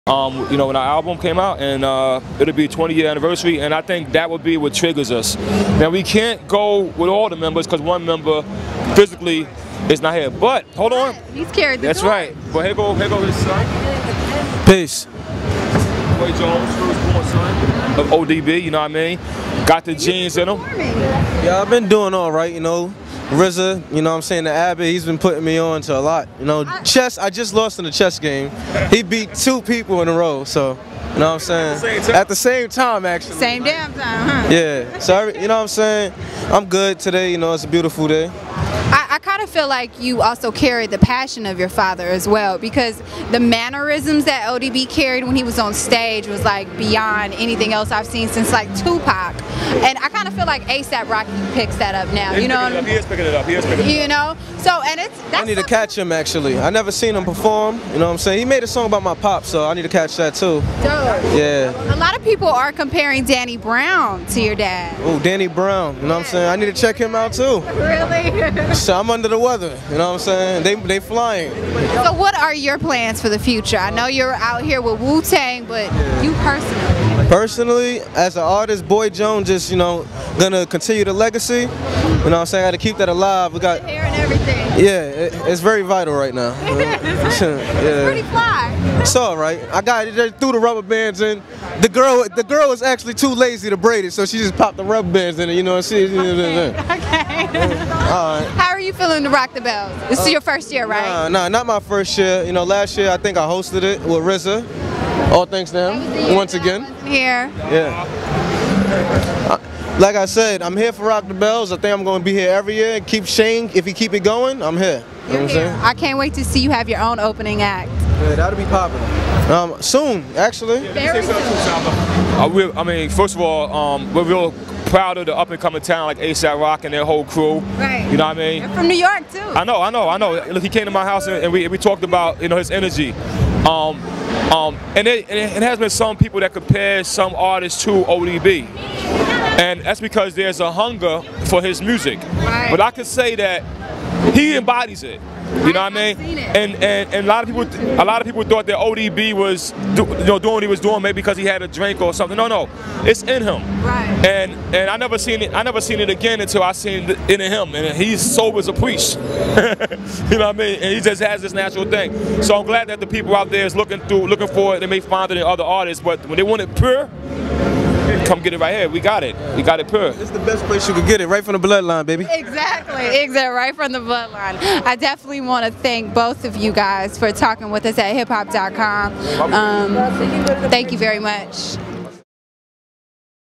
RZA and Boy Jones Interview with Saba G at RTB 2012
Part 2 of Saba G's interview with RZA backstage at Rock The Bells in Shoreline, when on the subject of Wu Tang's 20th Anniversary, they talk with Boy Jones, son of ODB, who will take his dad's place in the crew's lineup.

Interview by Saba G (@sabatheG)
Produced and filmed by Kuddy Leblanc (@KuddyLeblanc)